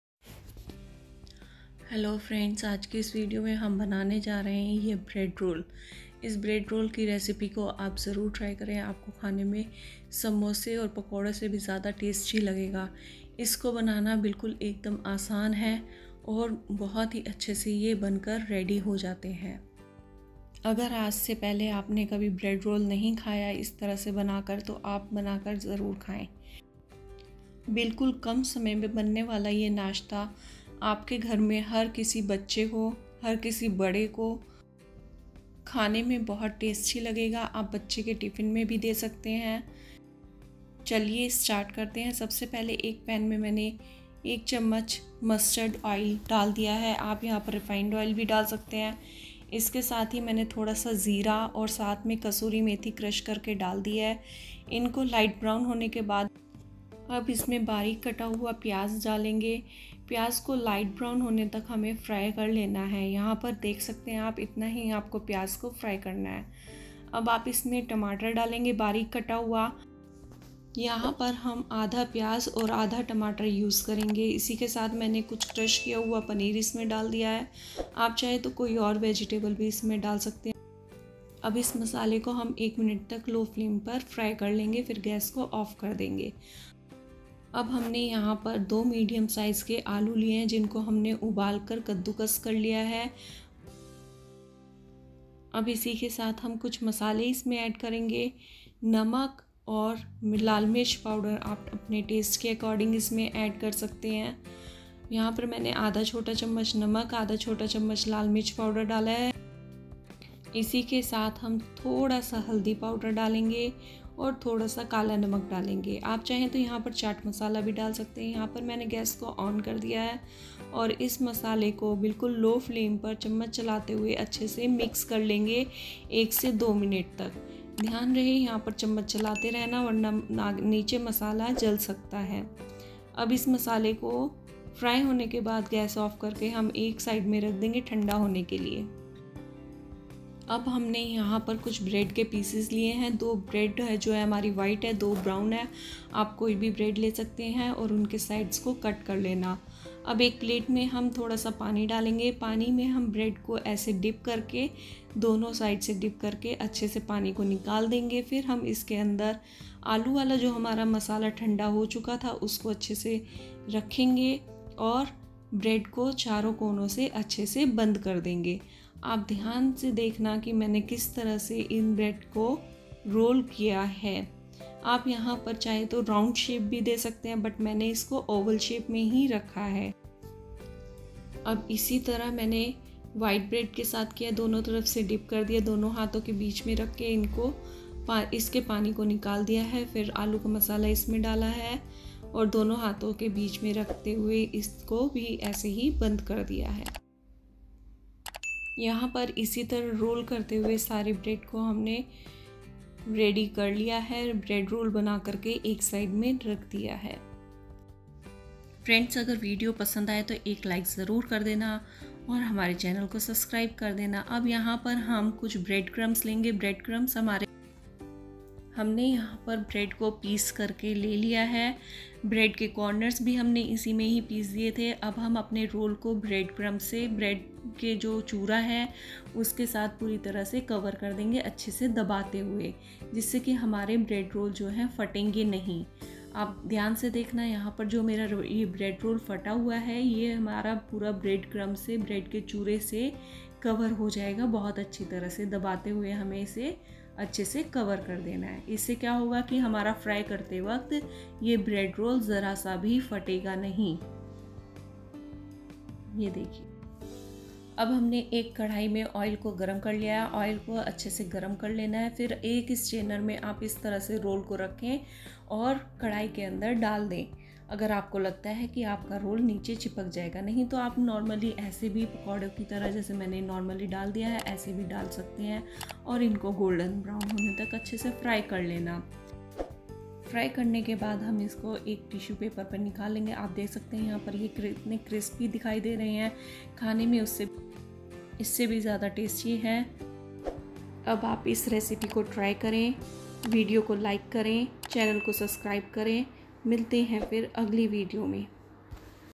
बिल्कुल आसान तरीके का नाश्ता/ब्रेड रोल की recipe😋Ramadan special||@AAS#like#subscribe
वीडियो में दिखाये गये अनुसार आप इस recipe को बनाये और खाए
इस recipe के लिए आपको कुछ ही समान की जरूरत होगी

bread toast ki recipe ☝🏻☝🏻☝🏻☝🏻